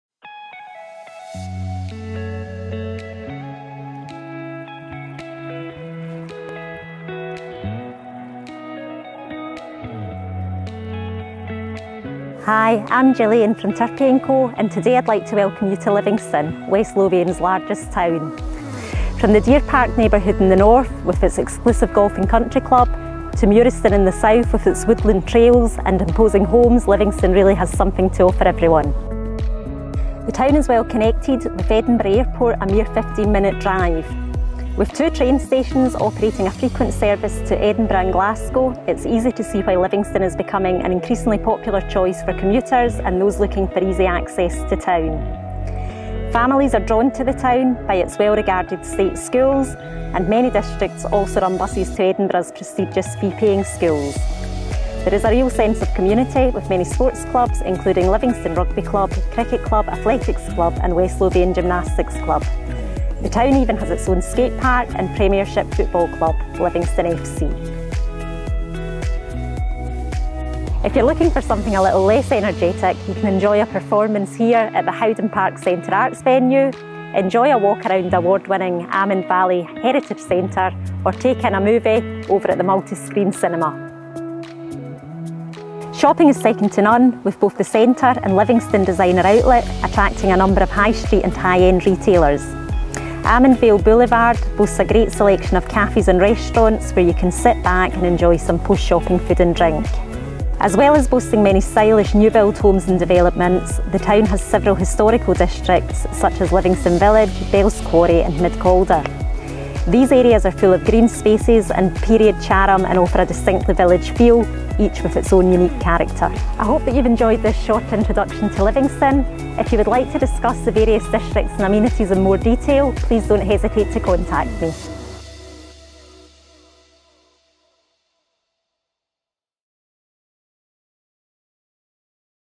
Showcasing Livingston
A short location video showcasing some of the main highlights of living in and around the Livingston area of West Lothian.